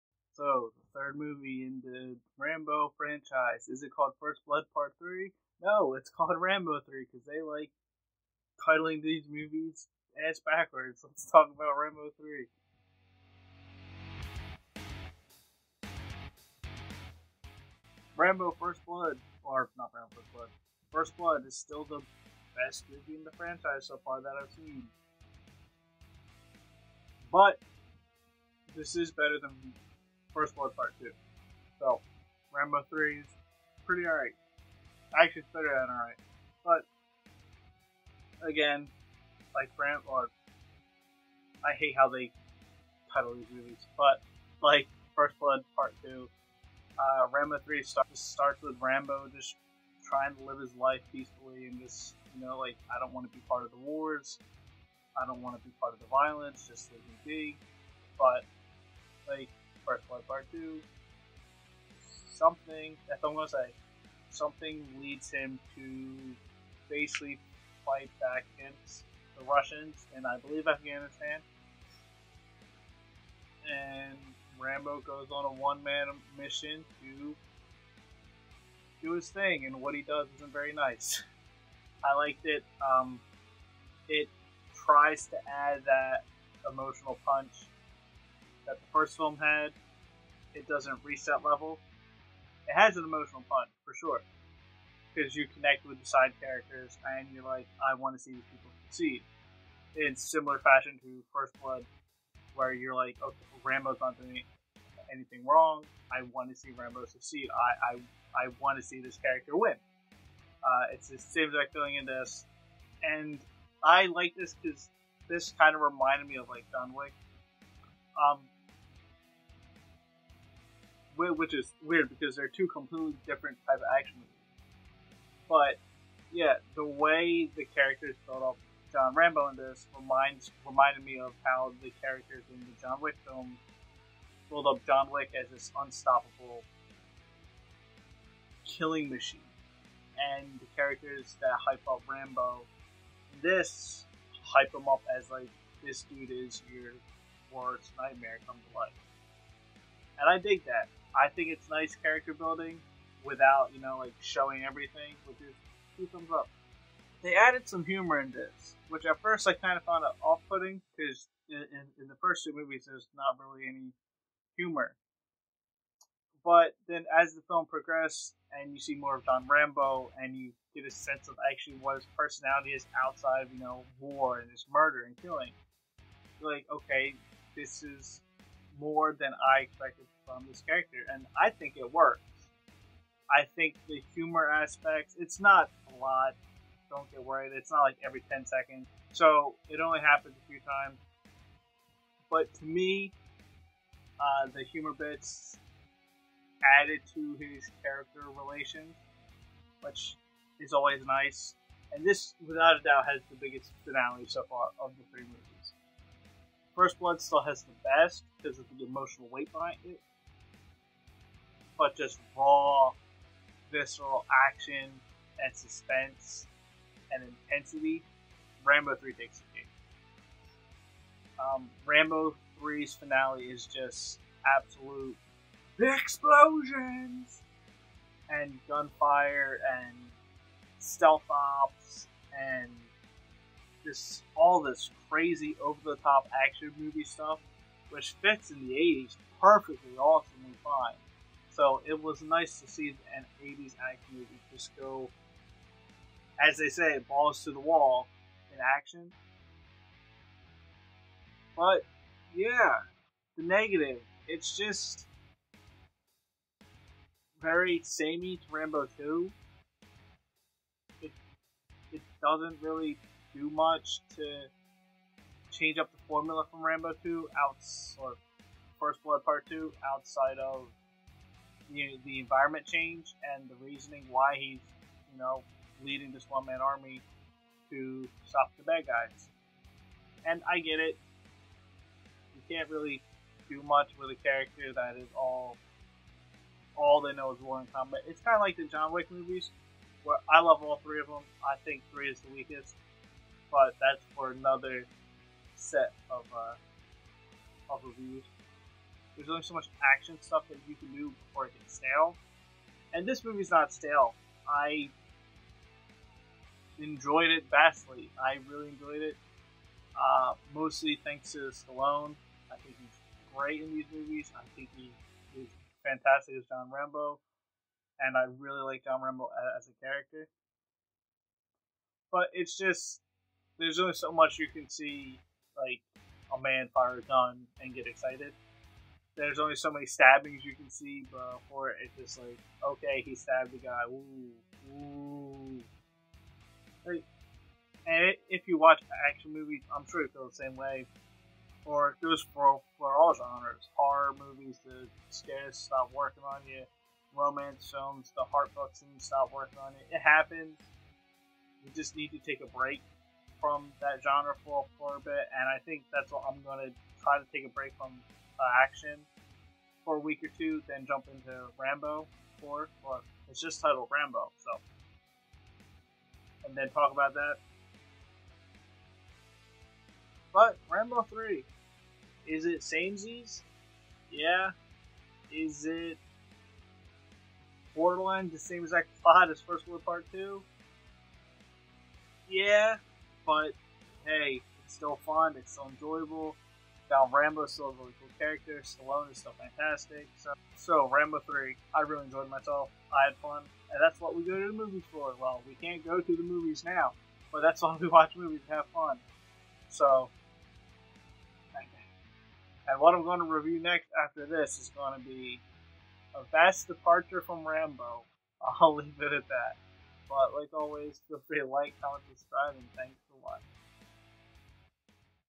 Rambo 3 - Movie Review
Rambo 3 has more explosions, veracity, and action. But does make up for lack of heart? Let's talk about it.